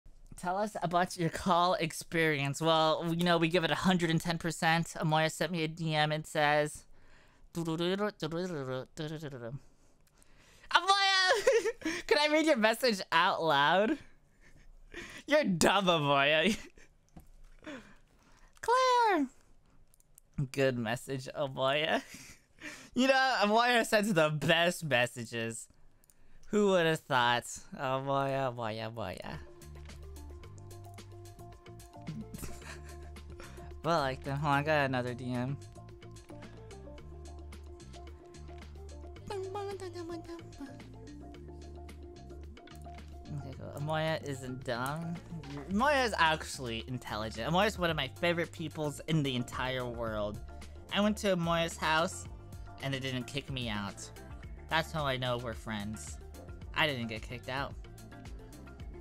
DM makes Nano smile
This is a clip from Nano Nano's Twitch stream entitled ❗️UNBANNED❗️WHAT IS THIS? ❗️BIG NEWS❗️ Femboy Guest @edenthefairy❗️I did NOT break ANY LAWS.❗️ which originally streamed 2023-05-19.

Clipped after the stream.

Nano Nano retains all rights to the stream content.